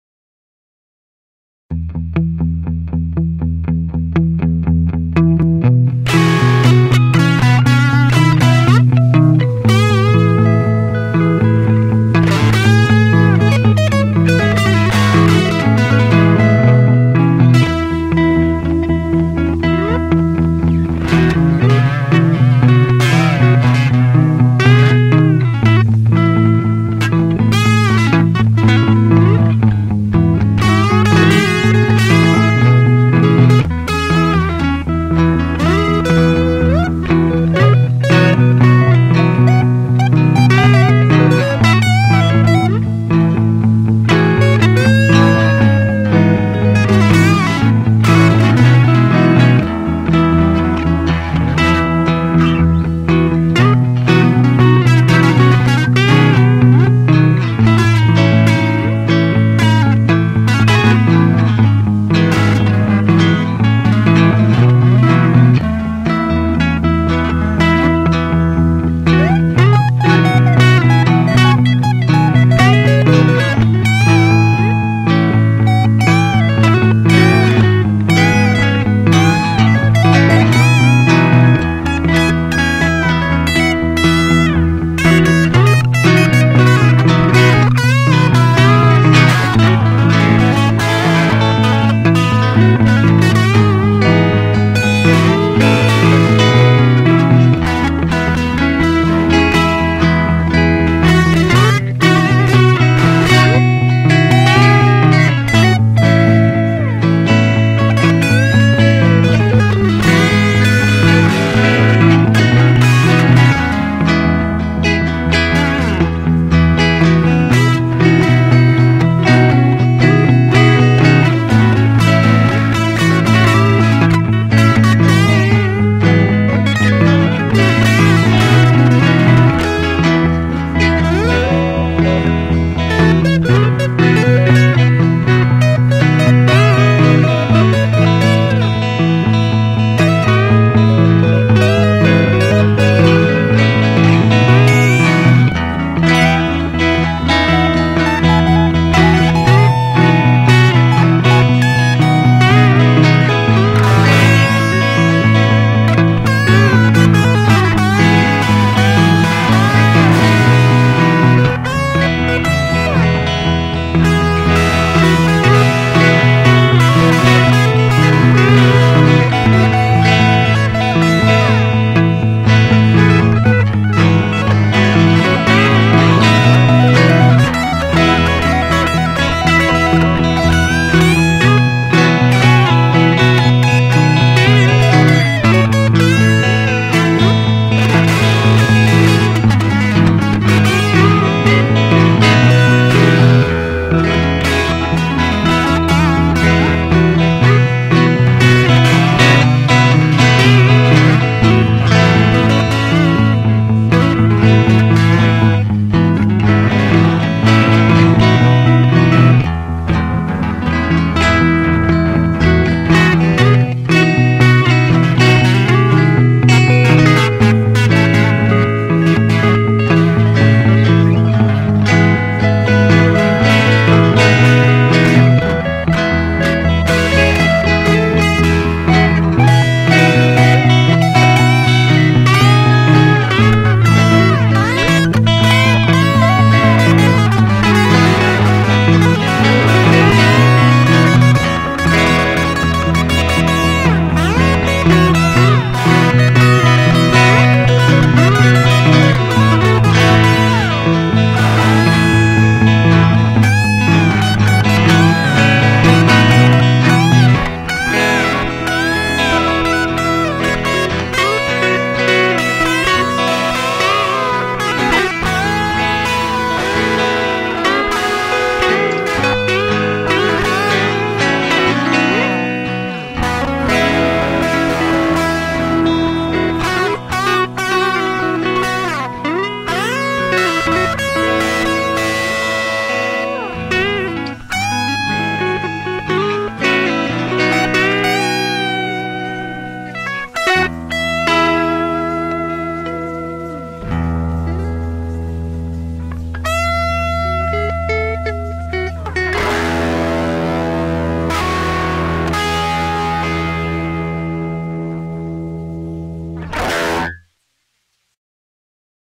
A Fall Melody
Came up with this melody on the bus ride home today